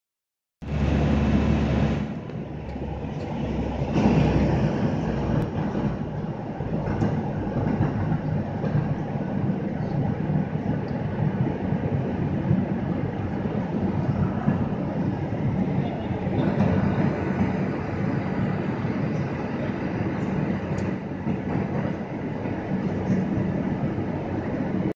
How clouds looks from Aeroplane | Airplane Flying on top of Clouds | Aasmaan ke Upar ki Duniya
Amazing view on top of Clouds from emergency window seat of Airbus 737